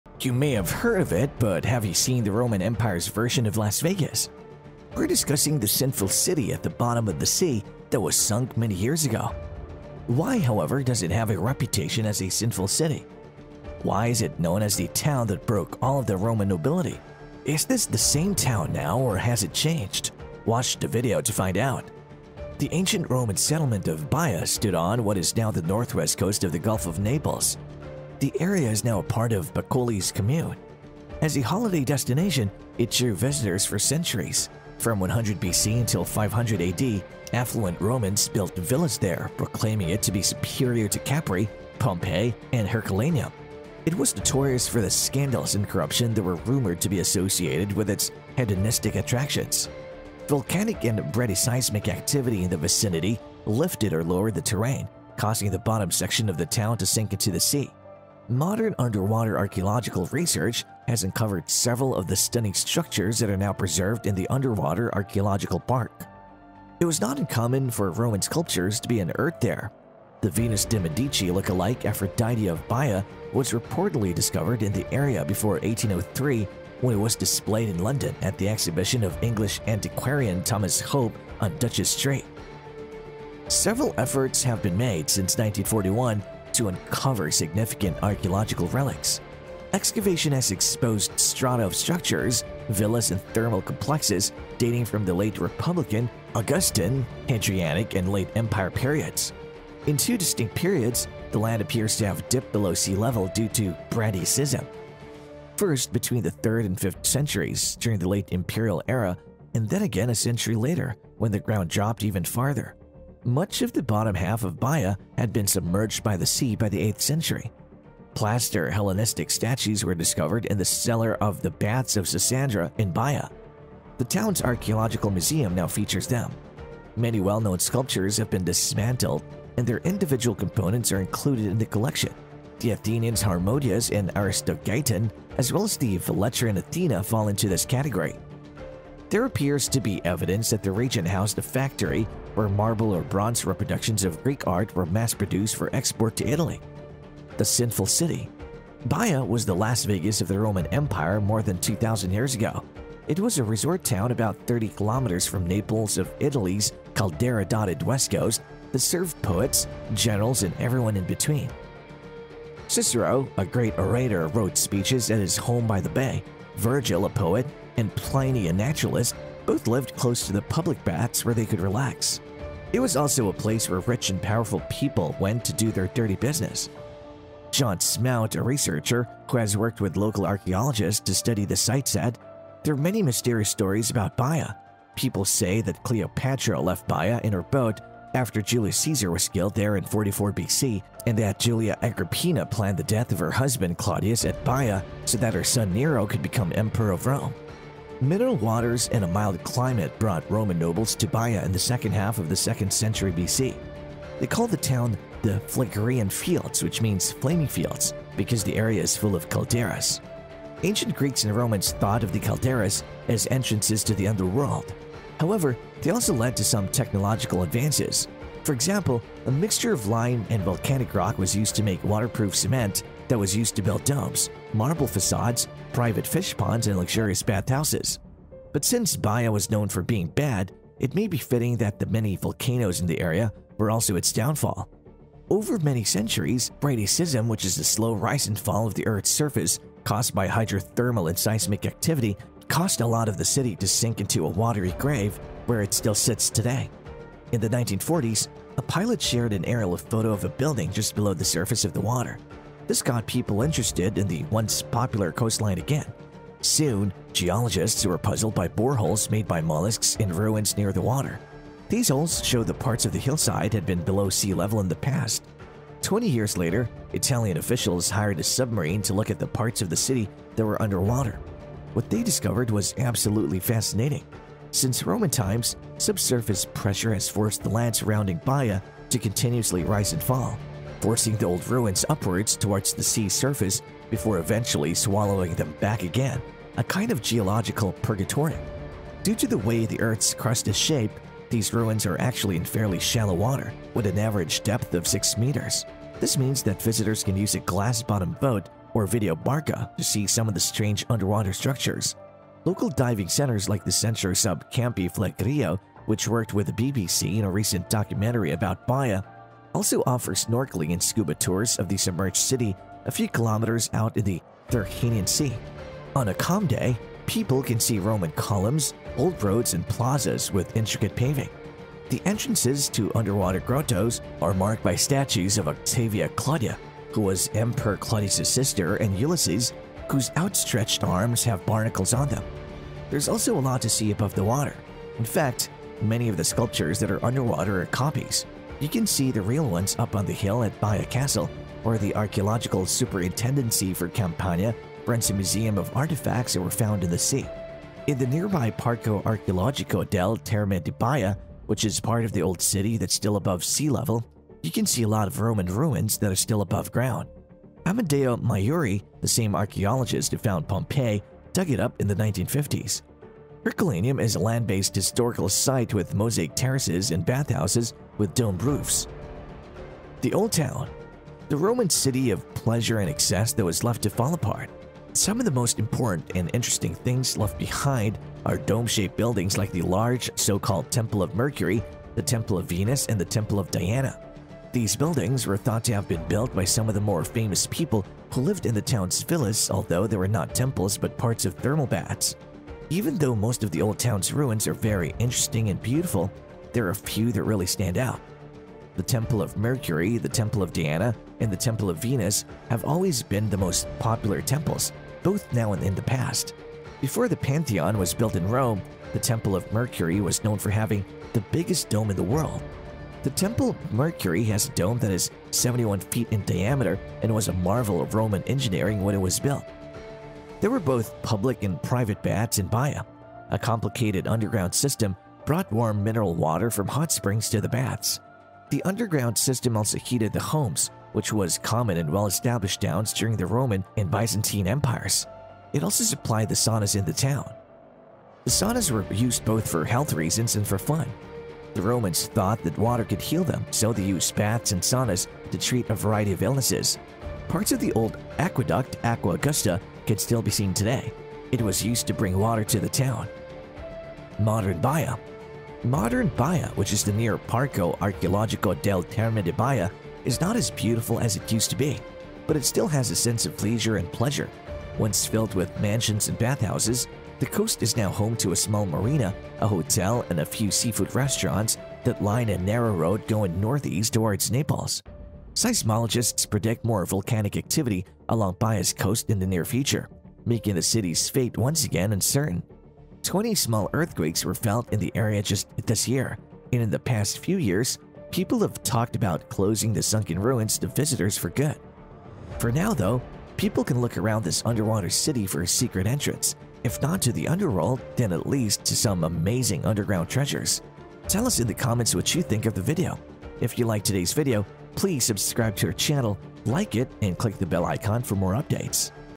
Dark History of Rome's Sunken Sin City - Baiae Ancient Las Vegas
Join us on a virtual journey through time as we explore the ancient city of Baiae, a once-thriving coastal resort town of the Roman Empire located near modern-day Naples, Italy. Known for its opulent villas, thermal baths, and stunning views of the Bay of Naples, Baiae was a popular destination for the wealthy and influential during the height of the Roman Empire.

In this video, we'll take you on a tour of Baiae's most iconic landmarks, including the Temple of Mercury, the Nymphaeum of Punta Epitaffio, and the Piscina Mirabilis, an ancient water cistern that still stands today. We'll also delve into the fascinating history of Baiae, including its rise to prominence during the Roman Empire and its eventual decline due to natural disasters and political turmoil.

Along the way, we'll hear from local experts and historians who will share their insights into Baiae's rich history and cultural significance. Whether you're a history buff, a travel enthusiast, or simply curious about the ancient world, this video is sure to transport you back in time and leave you with a newfound appreciation for the wonders of the ancient city of Baiae.